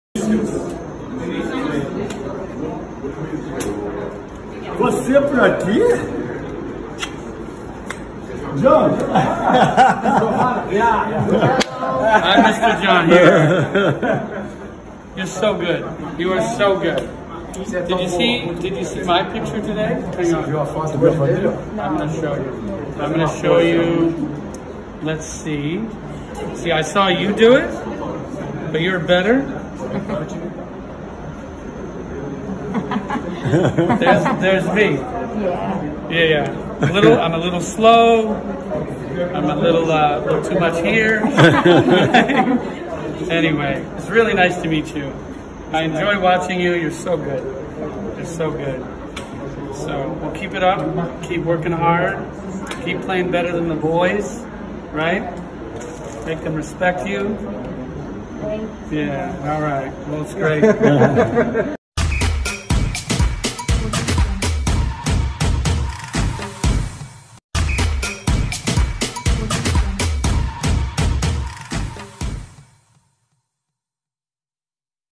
GIOVANNA, MAIOR REVELAÇÃO DO FUTEBOL FEMININO BRASILEIRO, COM JOHN TEXTOR, NOVO DONO DO BOTAFOGO!
Quem é Giovanna Waksman?